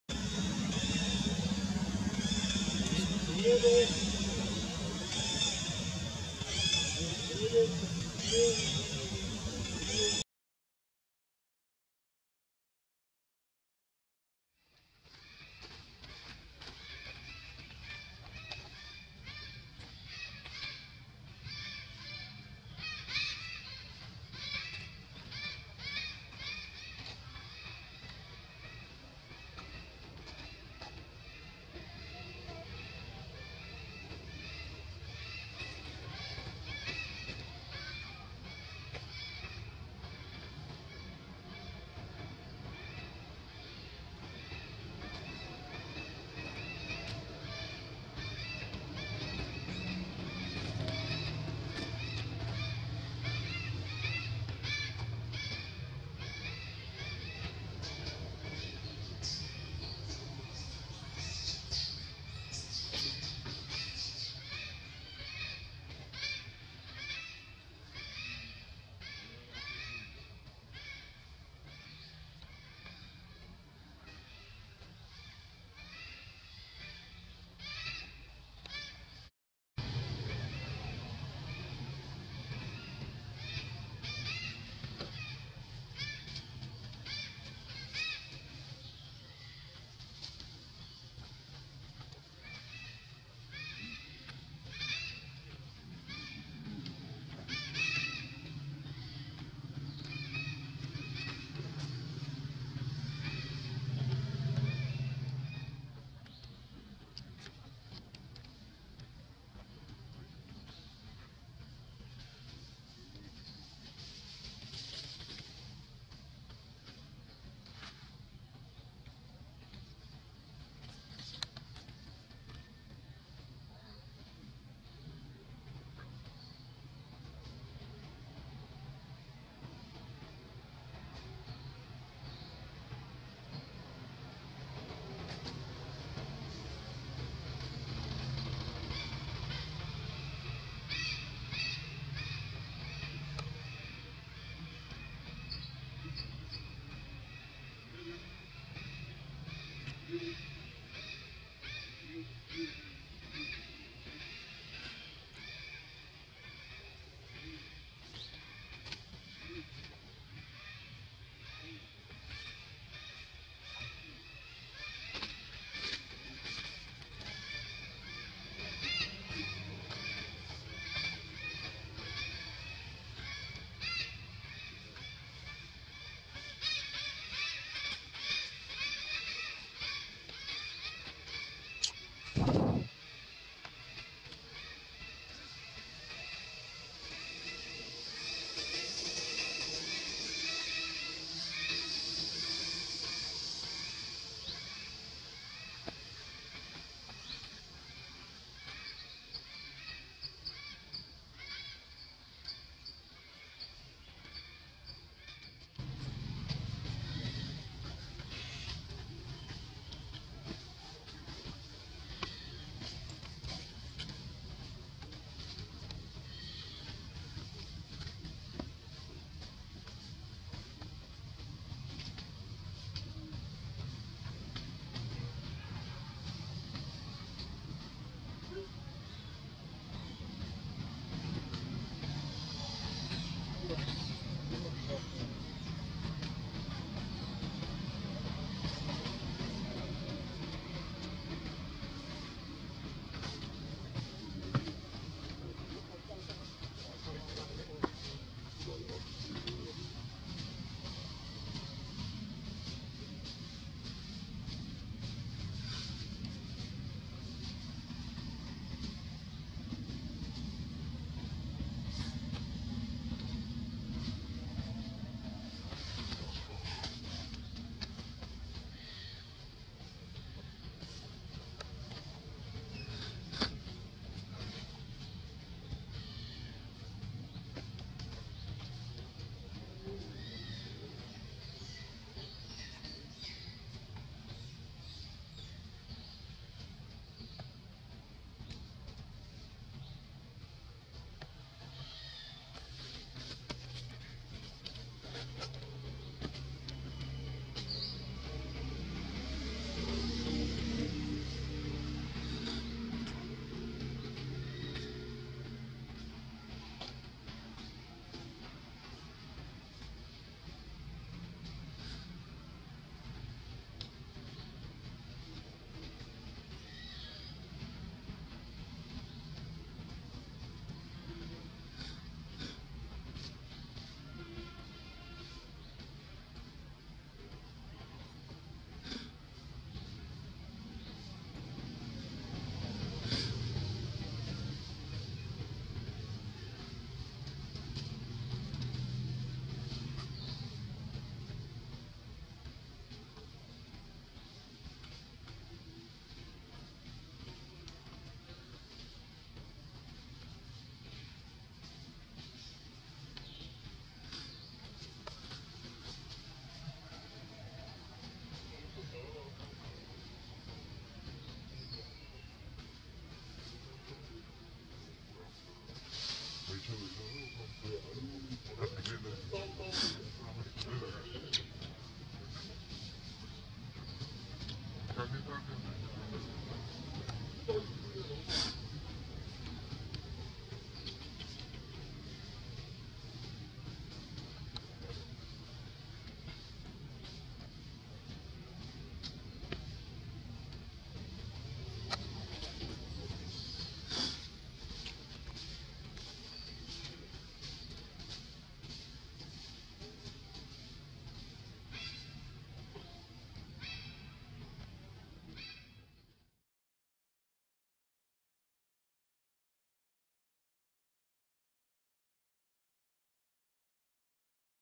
EXTREMELY DRAMATIC.!!TINY NEWBORN DITO TRYING TO HUG MOM SWEET,
Dear all Value Visitor, This Videos We Want To Show You About ,EXTREMELY DRAMATIC.!!TINY NEWBORN DITO TRYING TO HUG MOM SWEET,